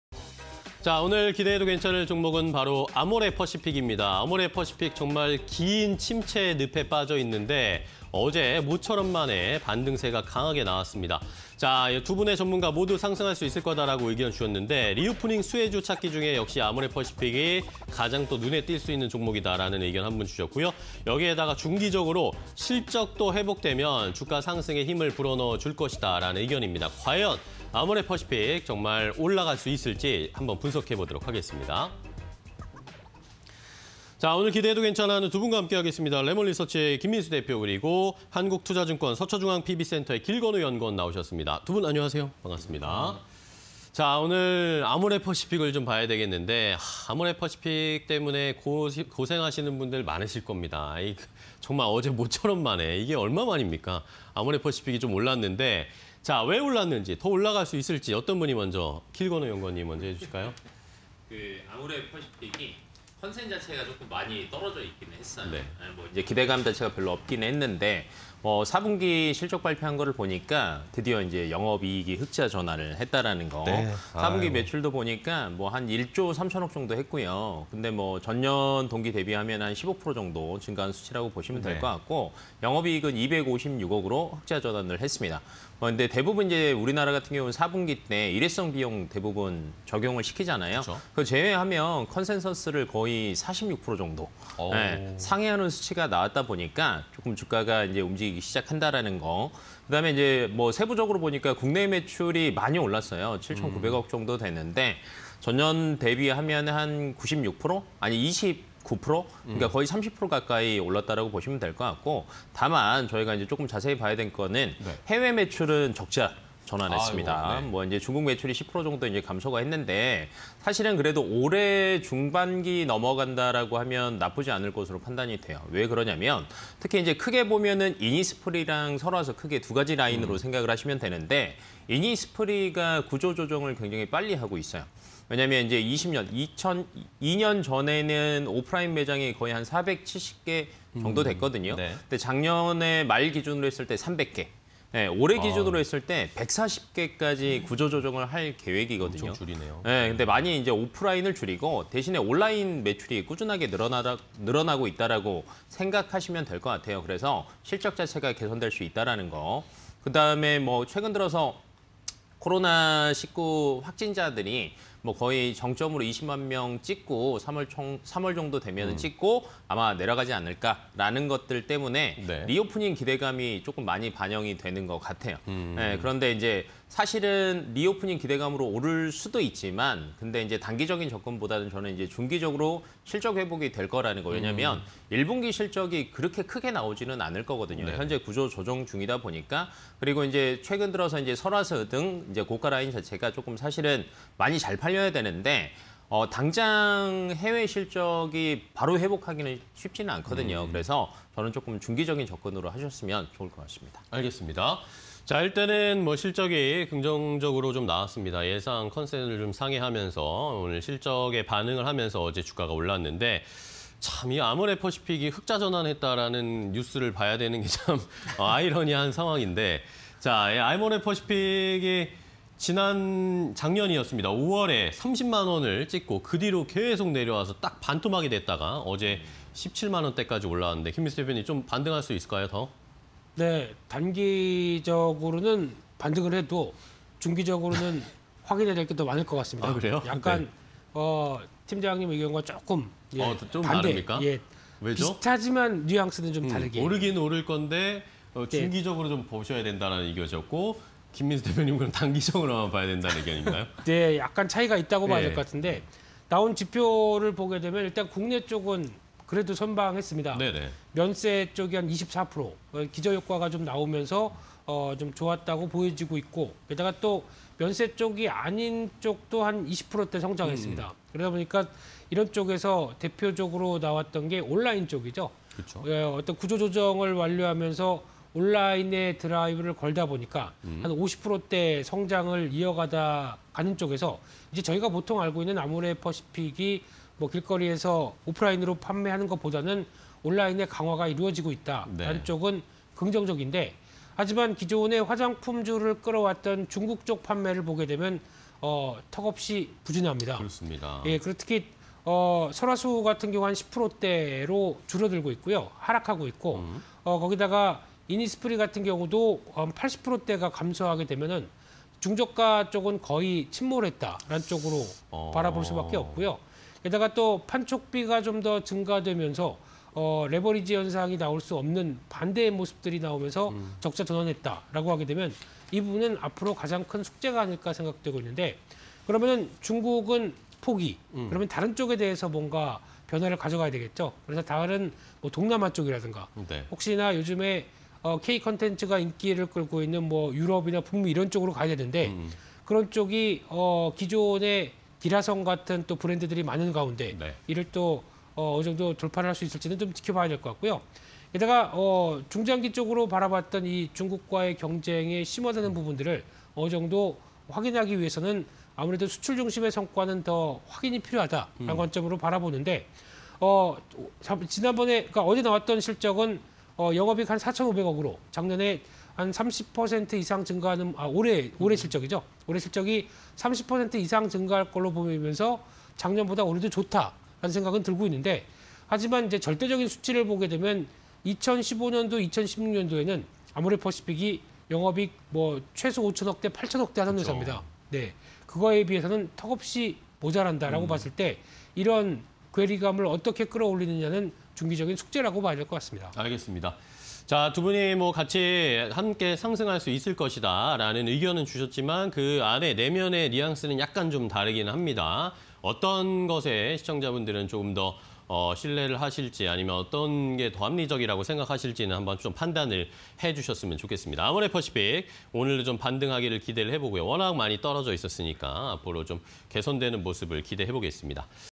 아모레퍼시픽... 리오프닝 수혜주 찾기, 중기 실적 회복 가능성_기대해도 괜찮아 (20220211)
아모레퍼시픽 더 간다
- 지난해 영업이익 흑자전환
- 국내 면세와 디지털 확대
- 고정비 축소에 따른 체질 개선 효과
- 中 이니스프리 구조조정 가속화
- 설화수 등 고가라인 비중 확대
- 중기 실적 회복 가능성으로 투자

- 면세 기저 효과
- 비면세 성장 전환, 온라인 고성장
- 中 판매 부진, 판촉 증가… 적자전환
- 상대 우위 성과 및 중국 업황 개선
- 다이고 영업 위축 및 대중국 경쟁 심화 여전
- 수출 중심 성과 및 지역 다변화 관건